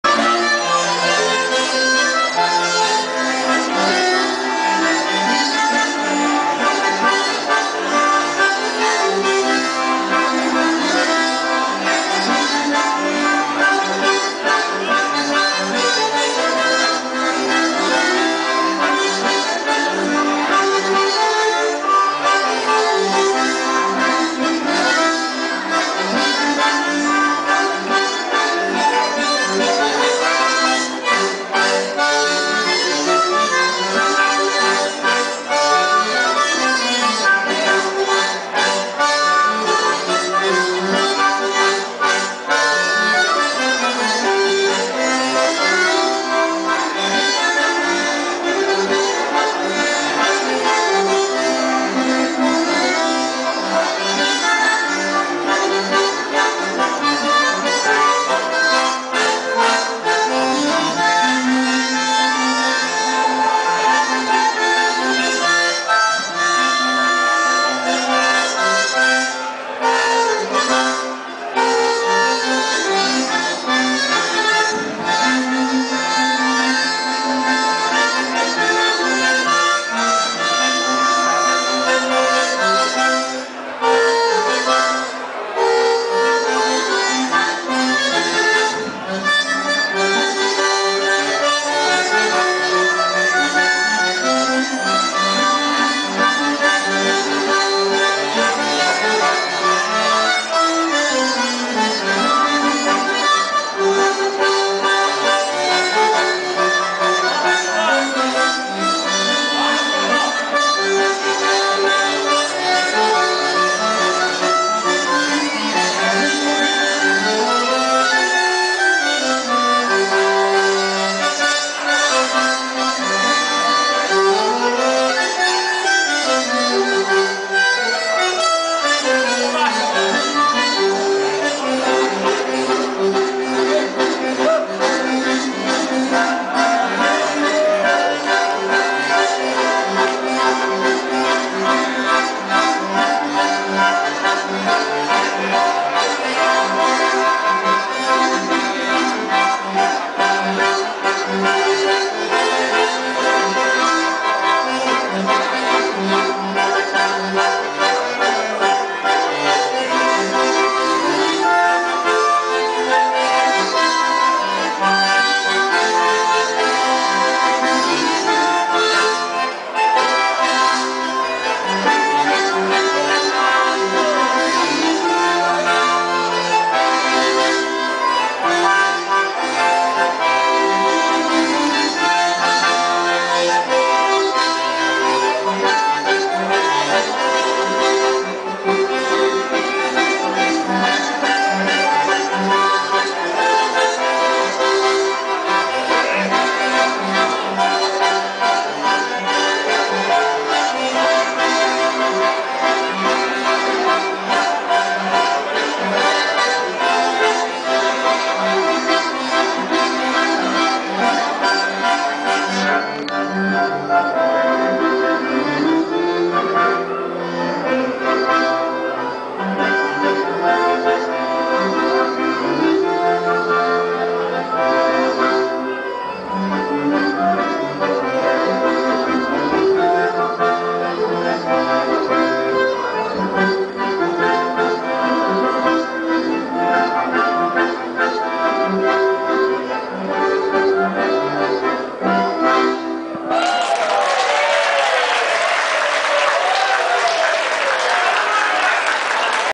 Bandonionfest, 180 anos de Imigração Alemã em Santa Catarina - Massaranduba 38
Bandonionfest, 180 anos de Imigração Alemã em Santa Catarina - Massaranduba 34. No palco os jovens Maikel Guckert e João Paulo Deckert, ambos alunos da Escola de Bandoneon do Botafogo Futebol Clube, Barra do Rio Cerro, Jaraguá do Sul (SC), cujas aulas são ministradas na Unidade do SESC Jaraguá, sob a docência do Sr Marcio Brosowsky, de São Bento do Sul (SC). A Escola do Bandoneon foi fundado em outubro de 2008 e o primeiro local de funcionamento foi o Museu WEG. Ademir Pfiffer - Coordenador